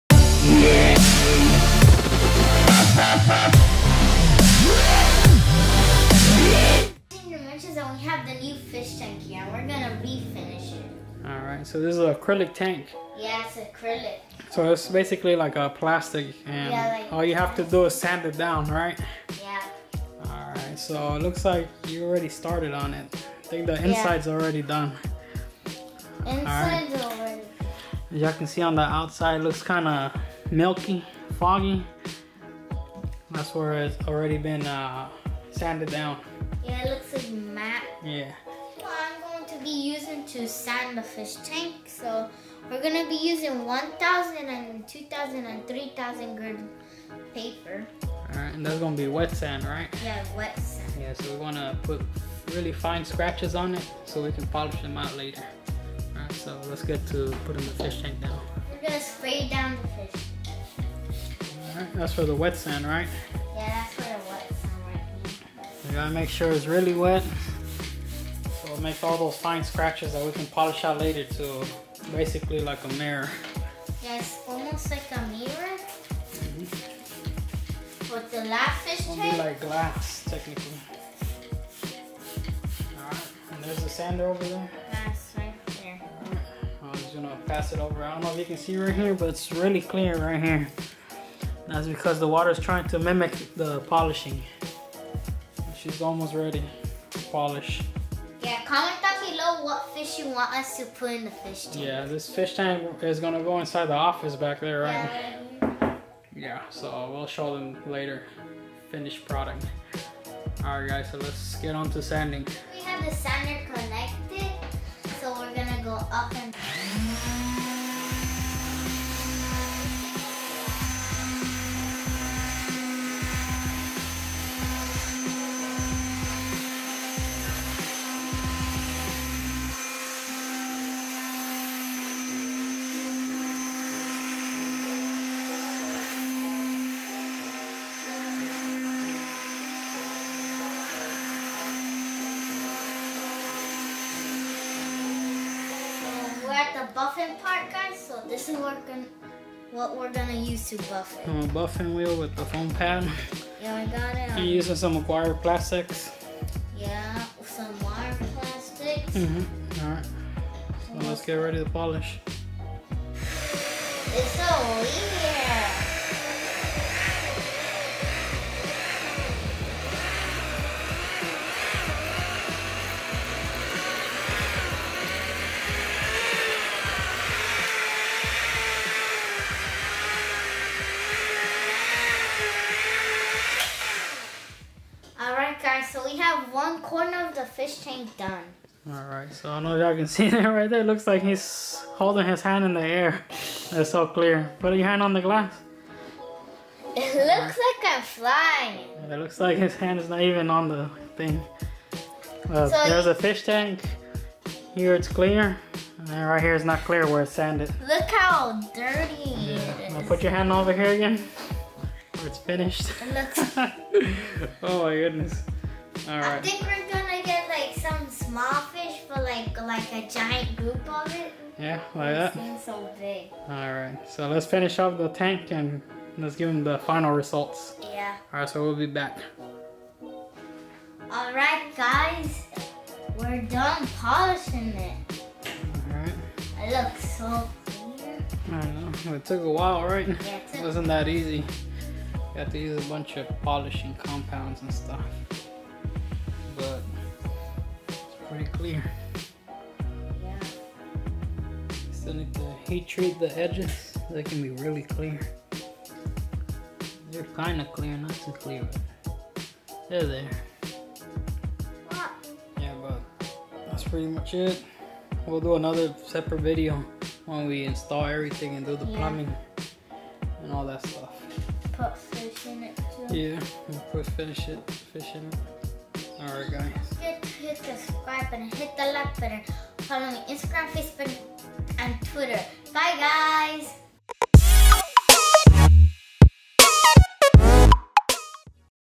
Refinishing our new Acrylic Fish Tank
So we picked up this tank a while back down south in Galveston. Its was a bit rough in shape,  so we decided to refinish the whole tank again by wet sanding and polishing to a like new finish. Like always thanks for all the support guys, really appreciate it!